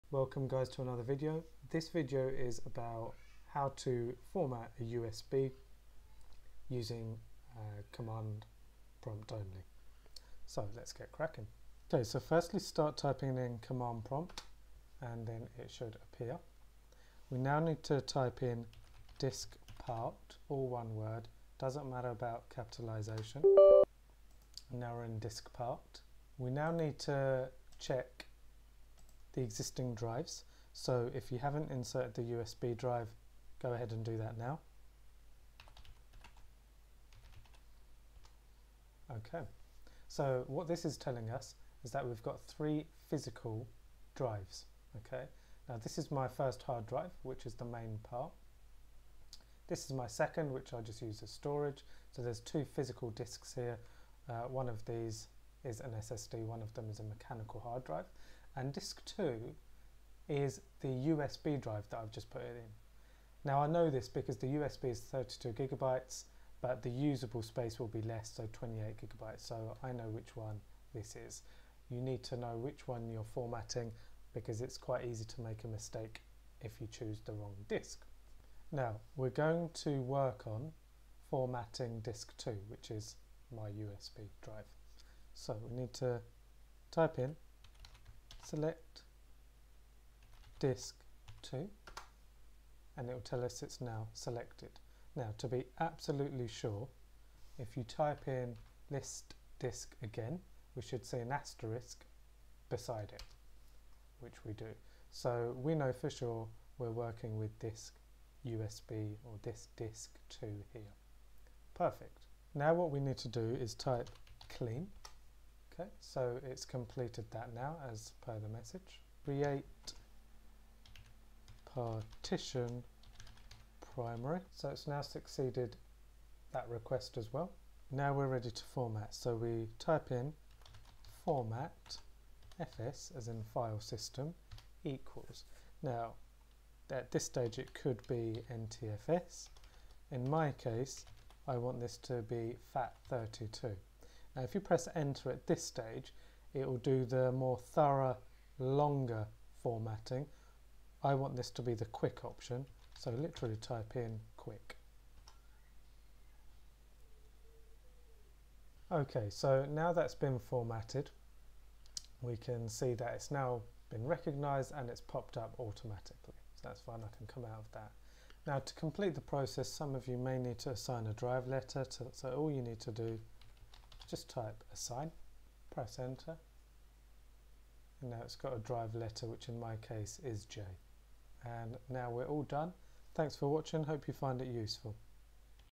How to format USB drive using Command prompt
Format USB drive/stick using command prompt. Once Command prompt loaded, we will use the following commands; diskpart, list disk, clean, create partition primary, format fs=fat32 quick and finally assign.